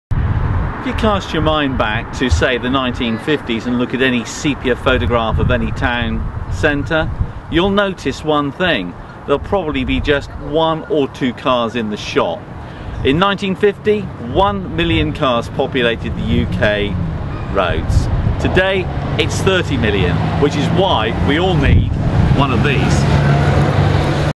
Cambridge Guided (missile) bus
My friend Robert Bond coming within millimetres of being hit by the Cambridge guided bus